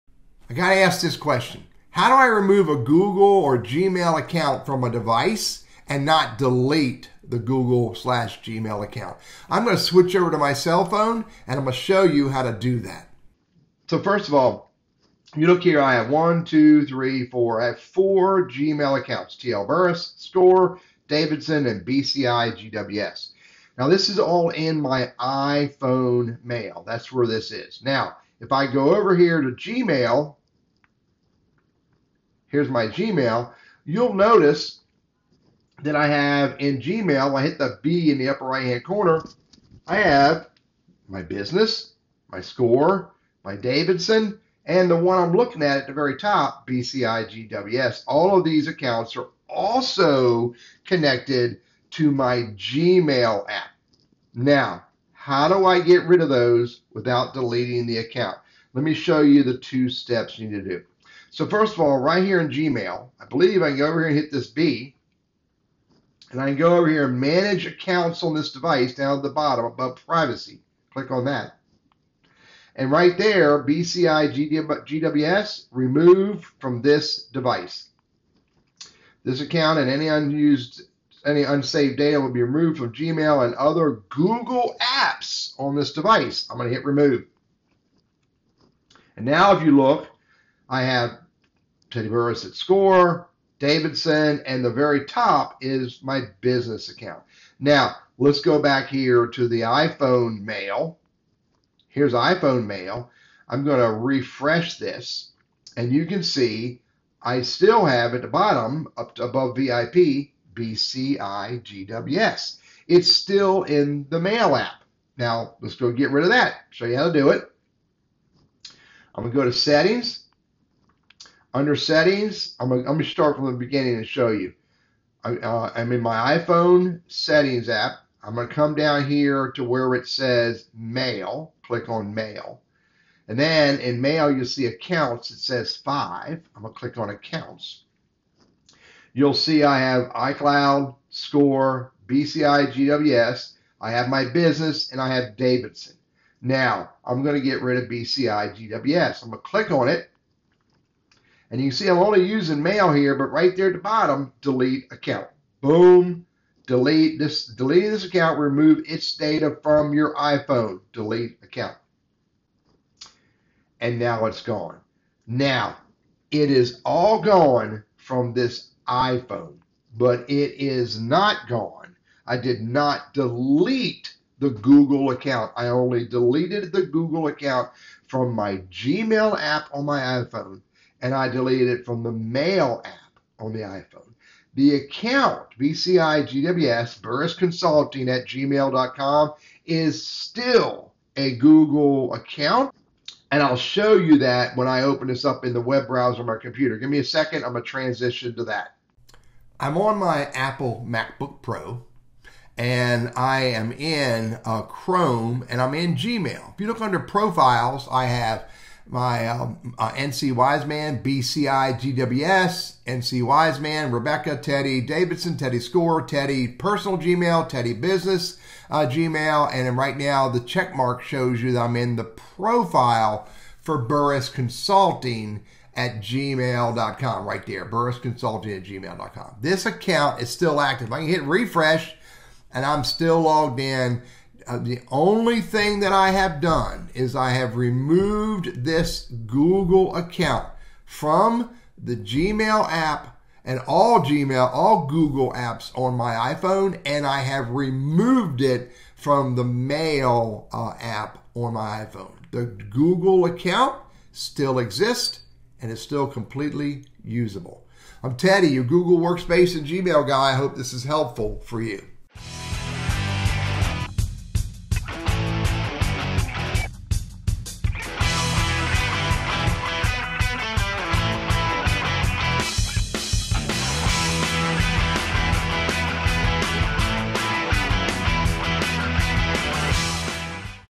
How do I remove a Google (Gmail) account from a Device and not Delete the Account?
I demonstrate removing a Google (Gmail Account from my iPhone without deleting the actual Google Account.